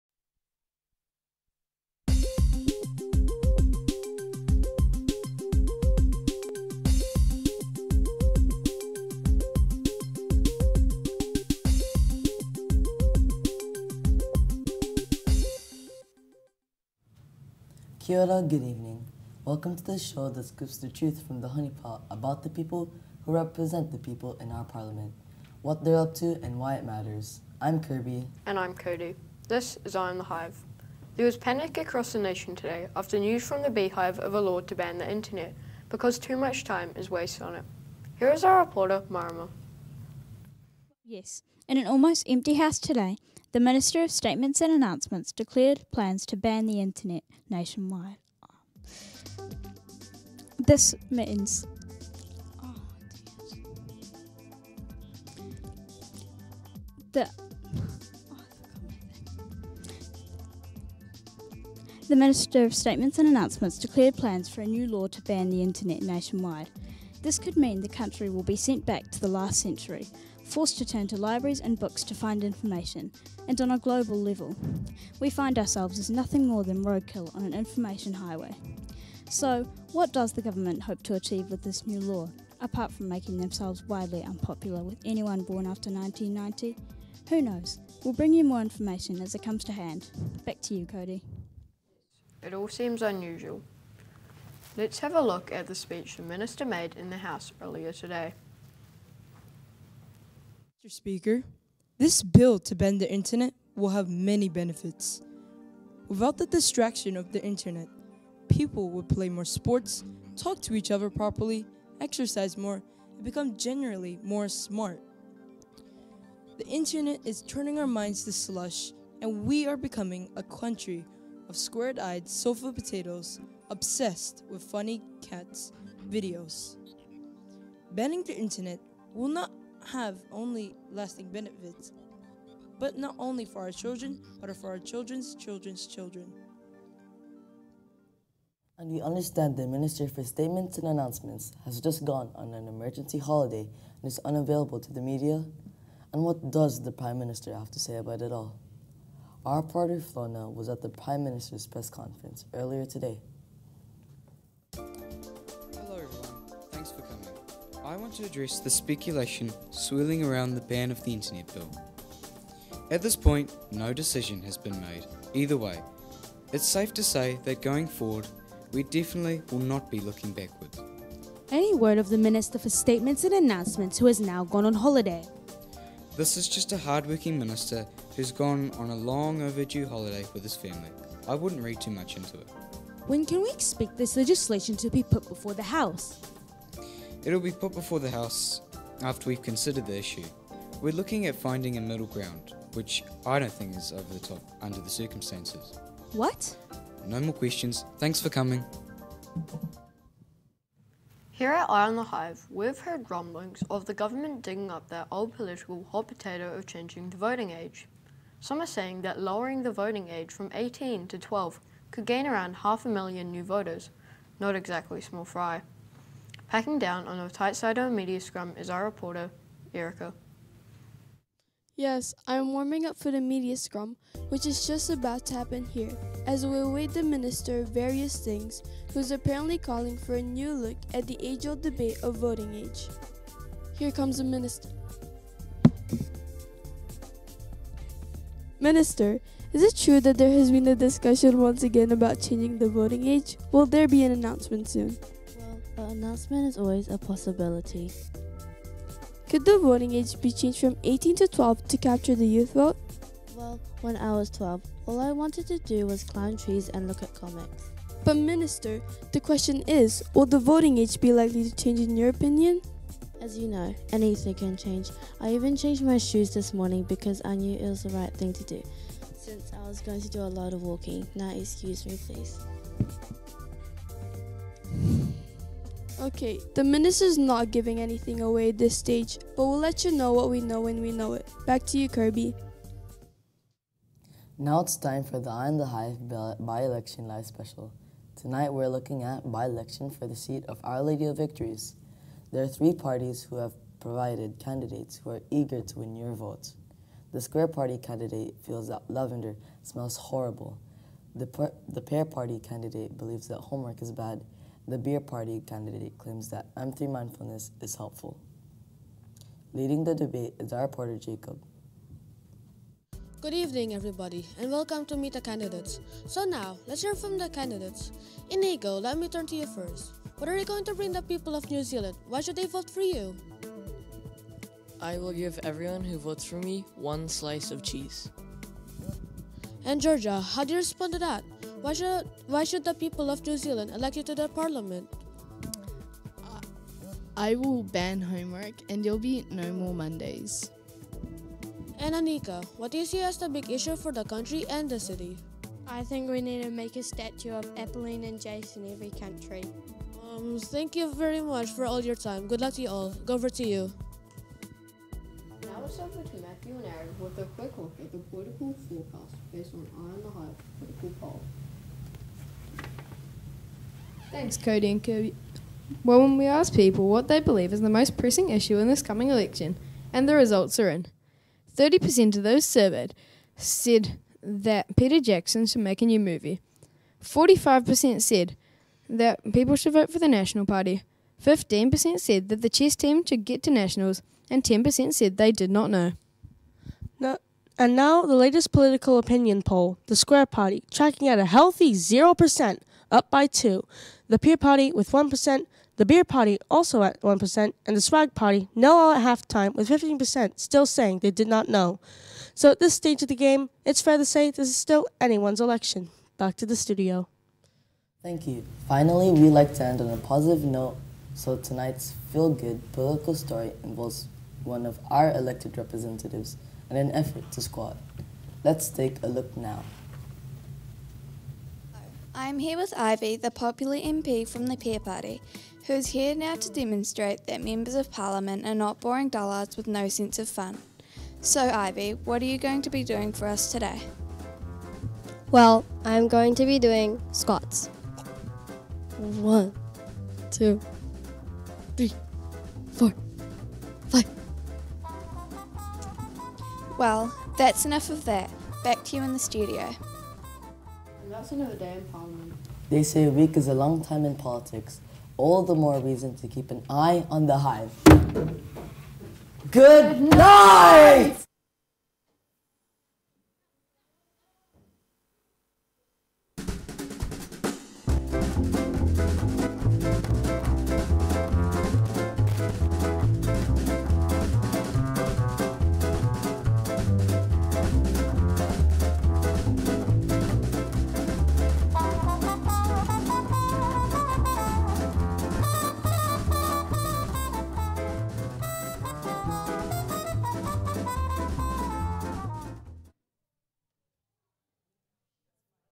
Our Lady of Victories School present EYE ON THE HIVE!
Our Lady of Victories School presents "EYE ON THE HIVE", scooping the honey from the political comb in search of the truth about the people representing the people in our government. Tonight: the Ban the Internet Bill - how will the people of the country react? We hear from the Minister behind the bill and also the PM's point of view. Also, a look at the by-election special - we hear from the candidates, before checking the temperature of the voter base in our EOTH political forecast... Recorded live in the Capital E OnTV Studio, on Thursday 29 September 2022.